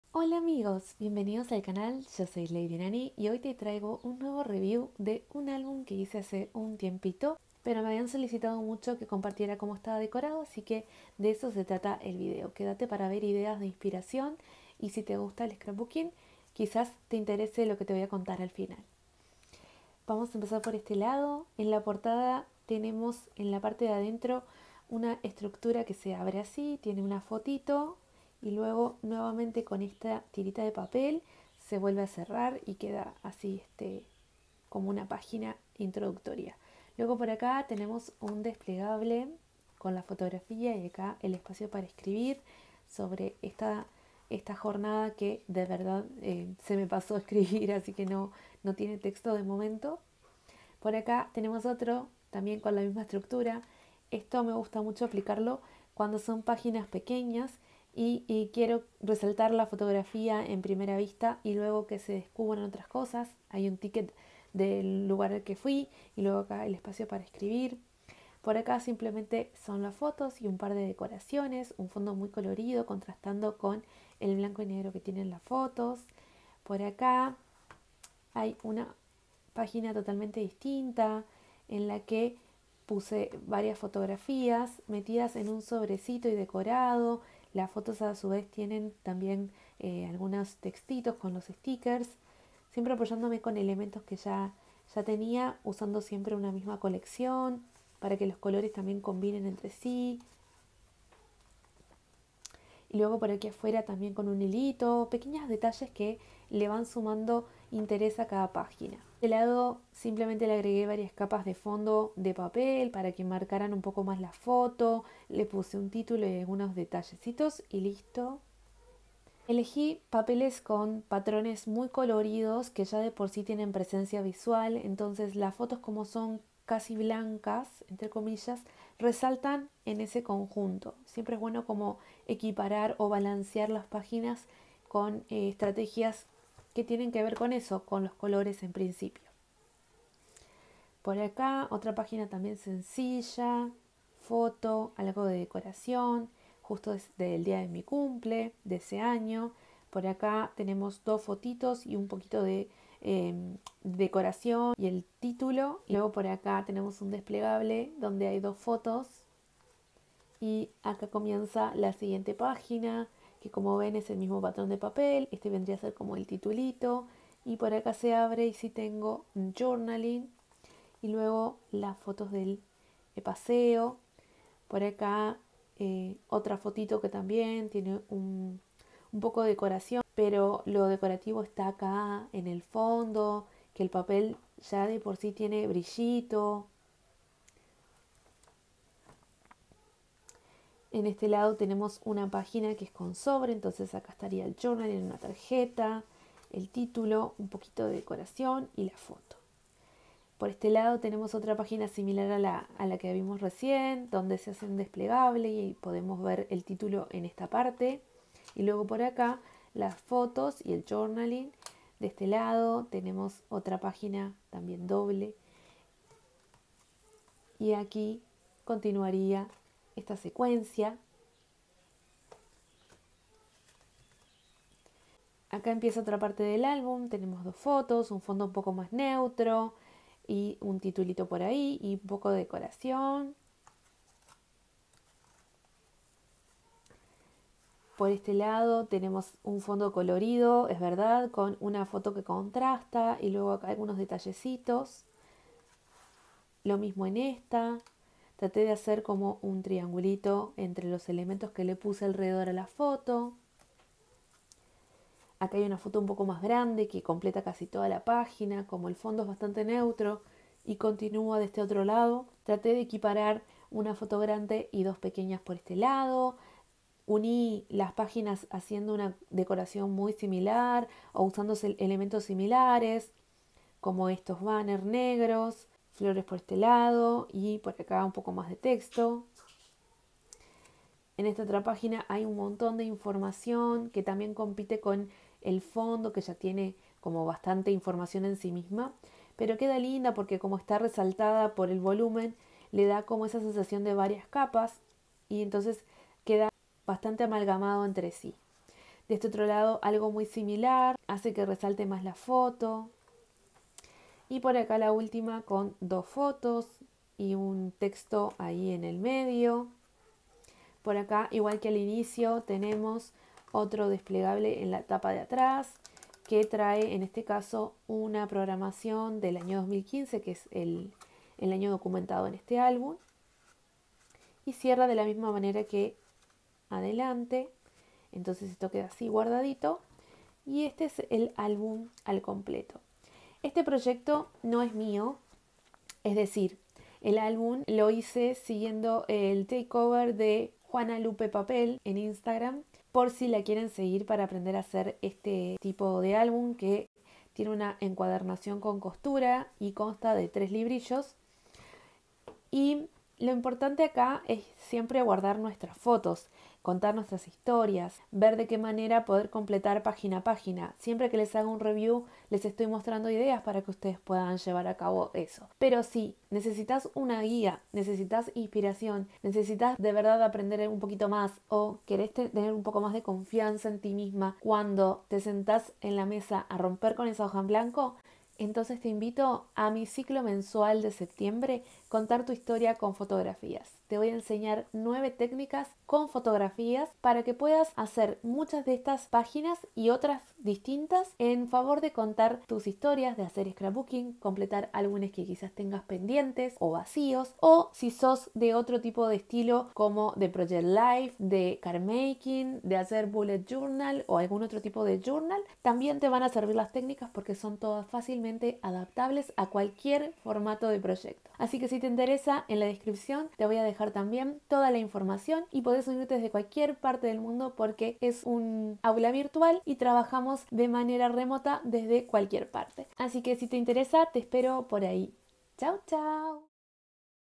Flip through Album 2015
Hola amig@s, bienvenidos al canal soy Lady Nany y en este video te enseño mi álbum anual 2015.

👩🏻‍💻 Si necesitas una guía, seguir inspirándote o aprendiendo 9 técnicas con fotos inscríbete a “𝐂𝐨𝐧𝐭𝐚𝐫 𝐭𝐮 𝐡𝐢𝐬𝐭𝐨𝐫𝐢𝐚” de Septiembre con fotos